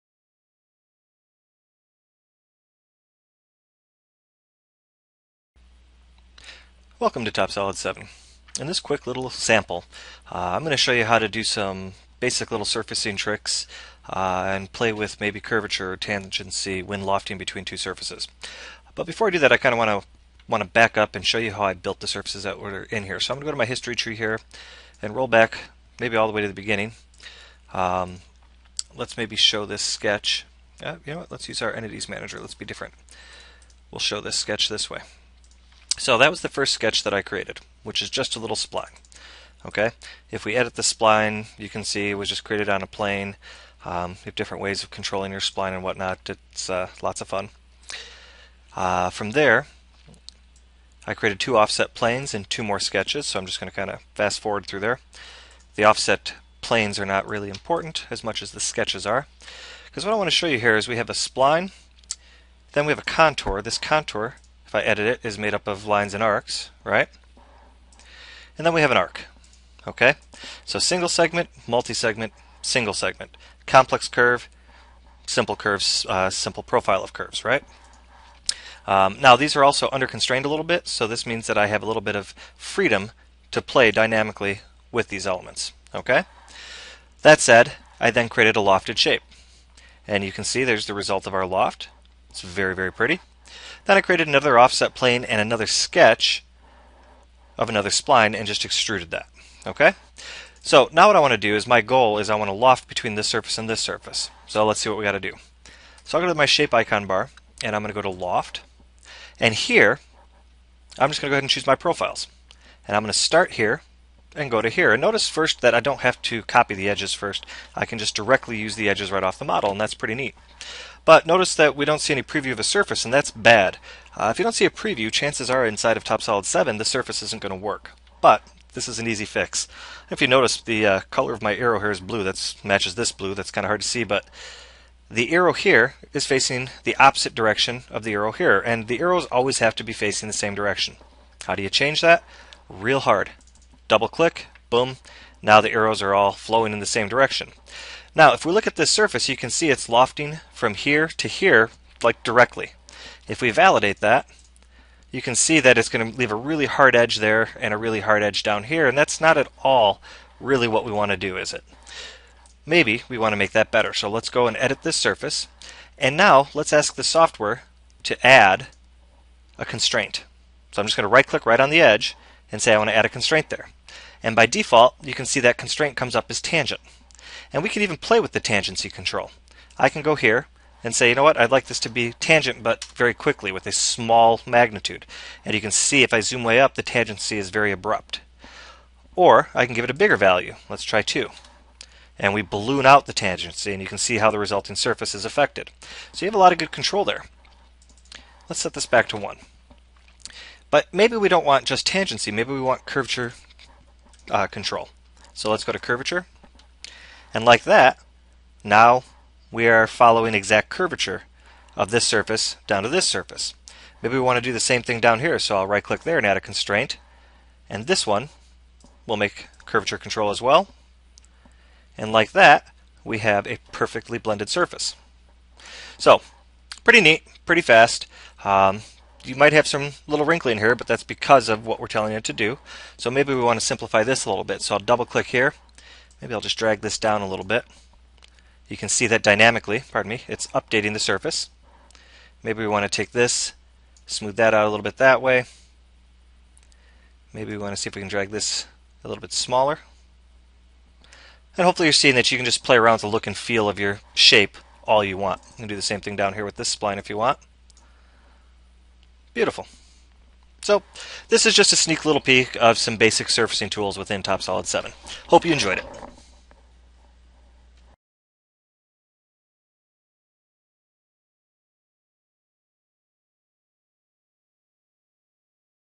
Lofting controls in TopSolid7
This video will introduce you to some of the lofting controls that you have in TopSolid 7